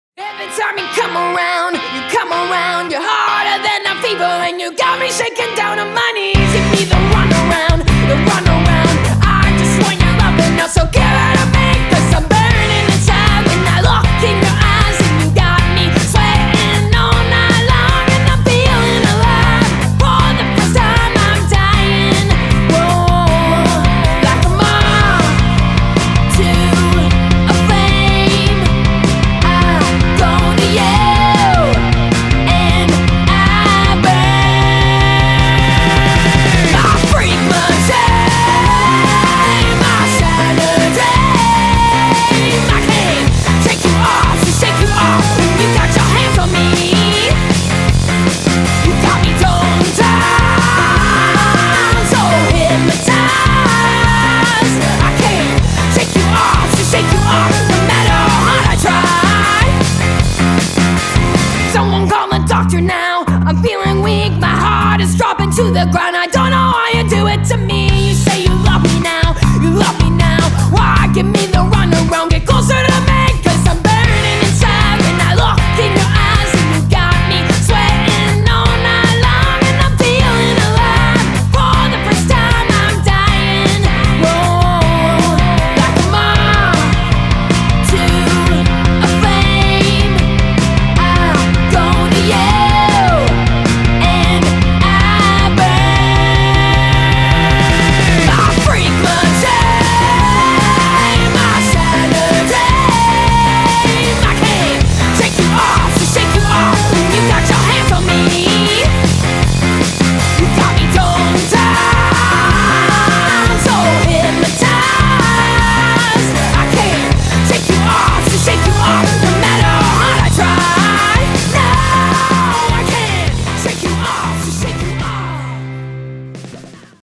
Fit For Rivals - Revolver Tour With Flyleaf, Diamante and The Agonist (Vlog)
Welcome to Freak Machine Records—an independent, artist-driven label dedicated to empowering creatives and delivering incredible music and more.